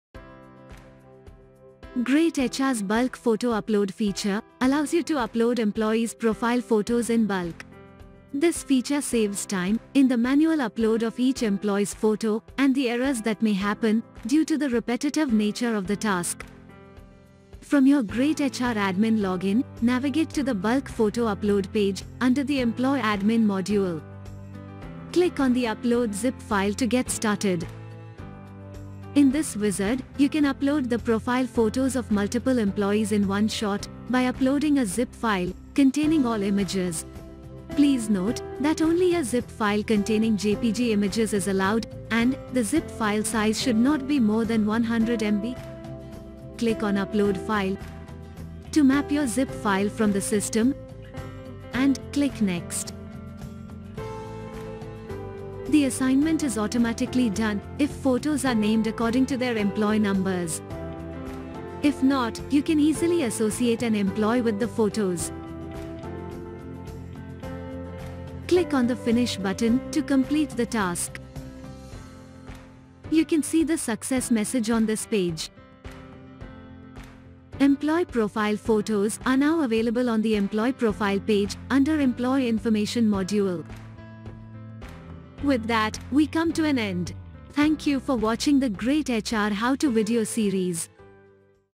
Upload Employee Profile Pictures in Bulk on greytHR
greytHR Bulk Photo Upload feature helps you to upload employees’ profile photos in bulk. You can upload the profile photos of multiple employees in one shot by uploading a zip file containing all photos on greytHR. Just rename the photos according to employee IDs, and the application automatically assigns the photos to the employees.

Read more about how to upload employee profile pictures in bulk